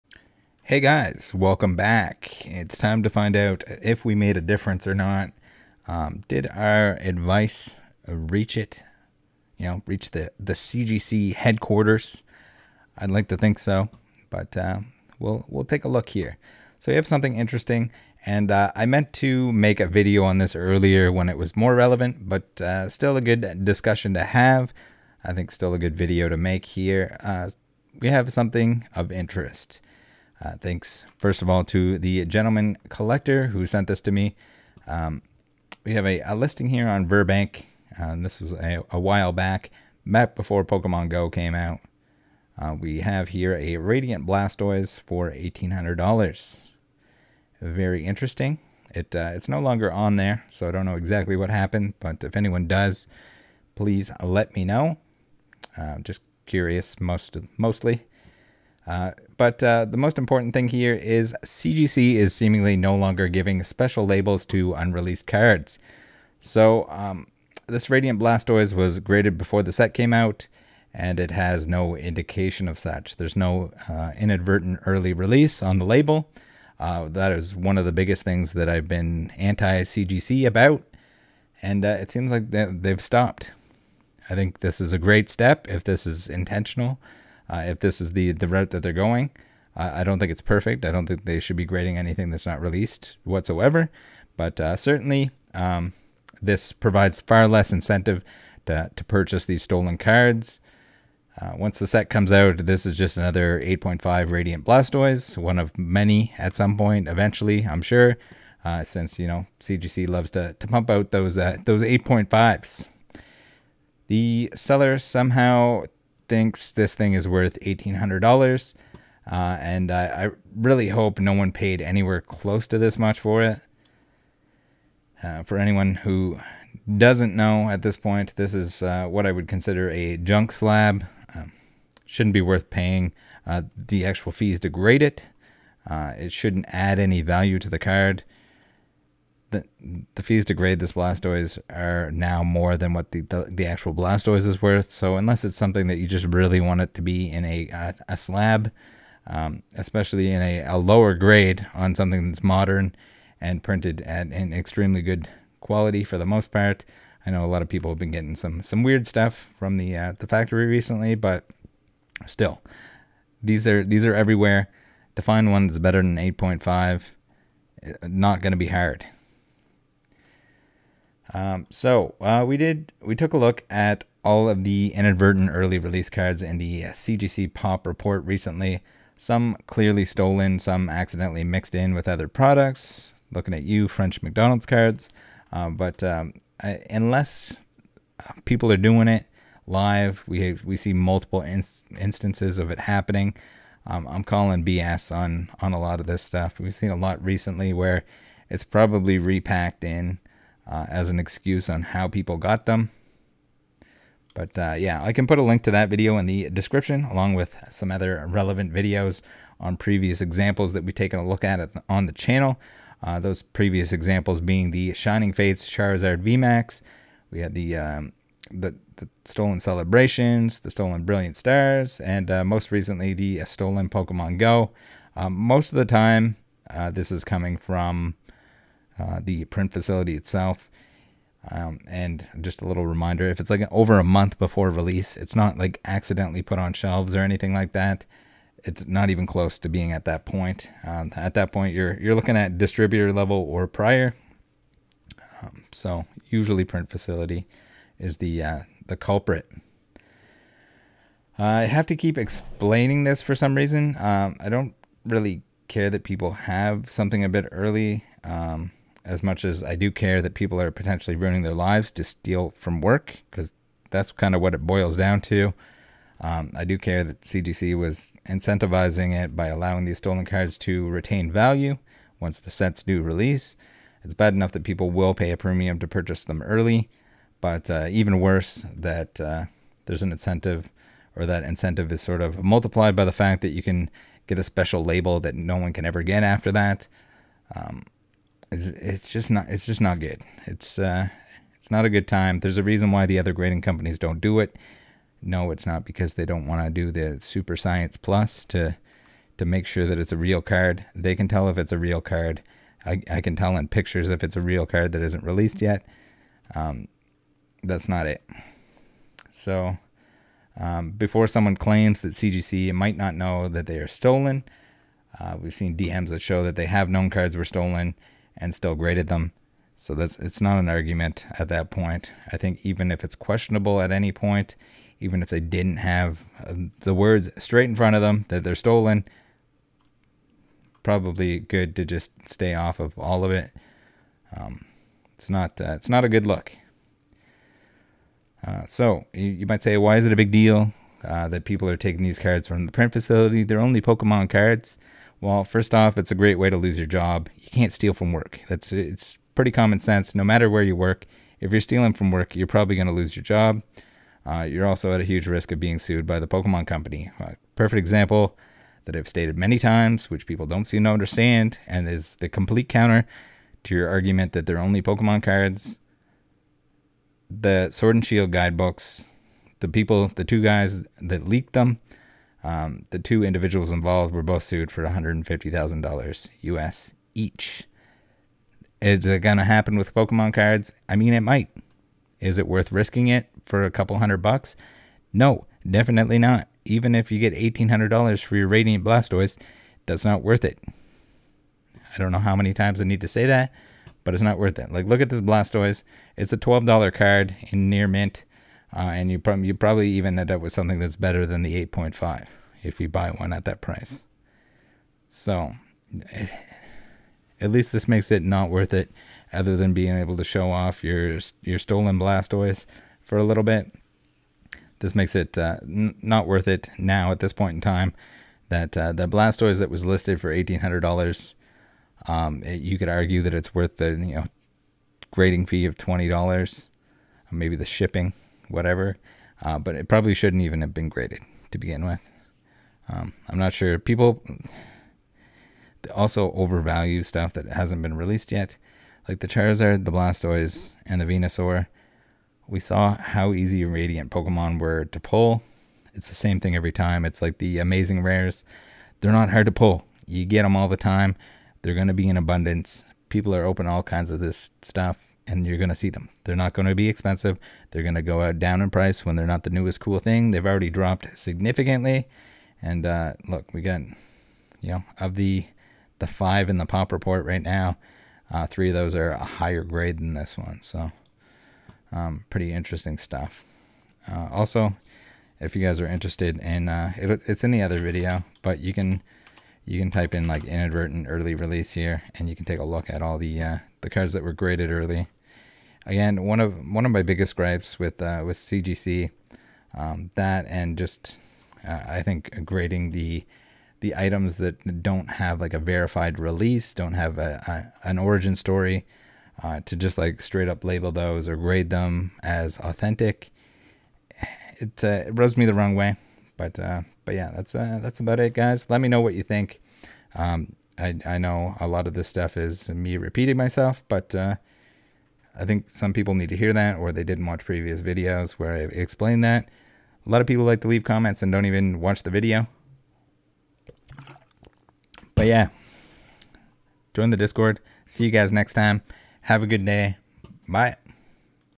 CGC Is No Longer Putting Inadvertent Early Release Labels On Stolen Pokemon Cards?
It seems that CGC is no longer putting the special Inadvertent Early Release labels on Pokemon trading cards before they are released. I think this is a great step to reduce the incentive to steal these cards from the print facility, distributor, etc...